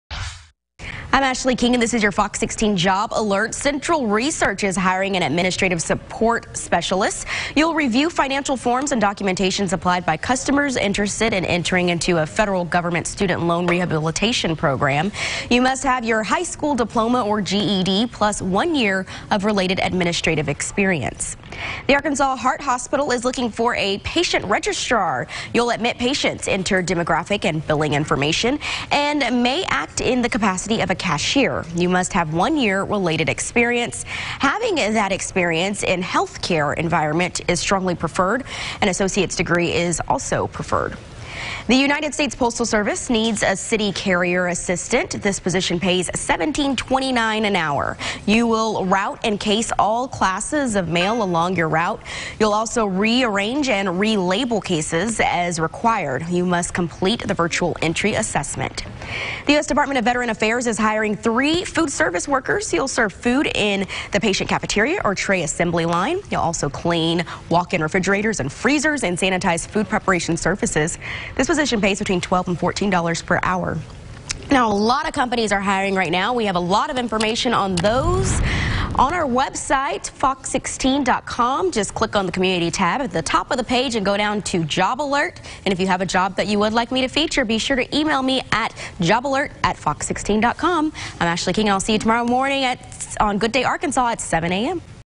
New jobs in Arkansas
Fox 16 stays overnight at historic 130 year-old Victorian haunted house in downtown LR